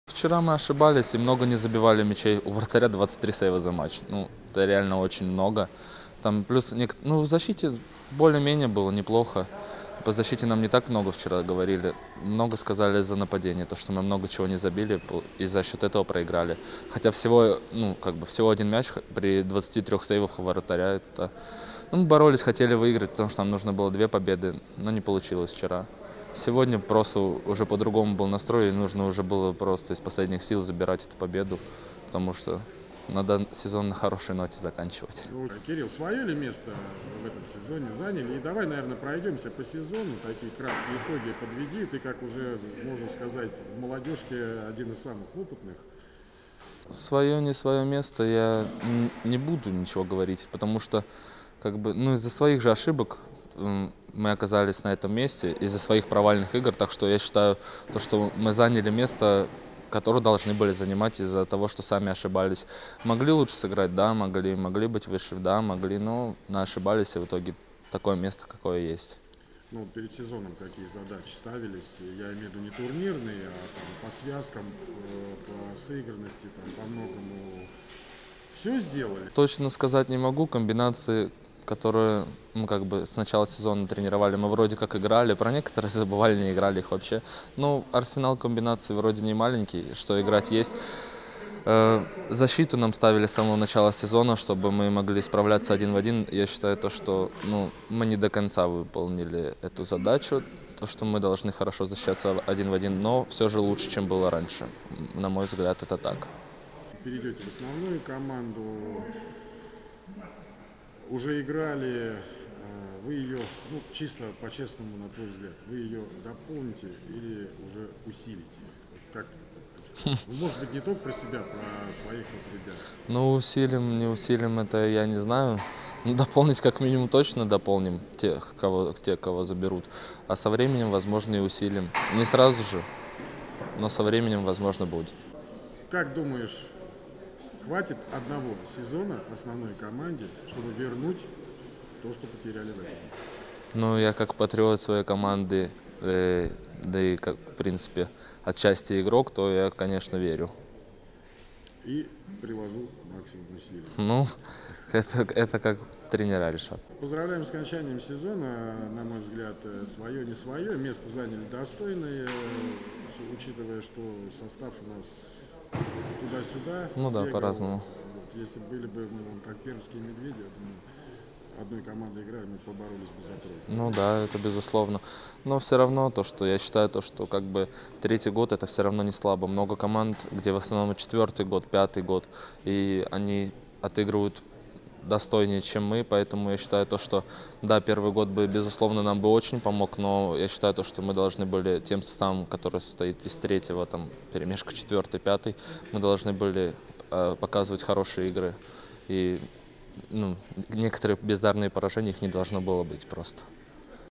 Комментарий Кирилла Терихова
15 мая 2022 г.

Комментарий по заключительным матчам против МССУОР №2 и краткие итоги сезона одного из лидеров молодежной команды волгоградского «Каустика» Кирилла Терихова.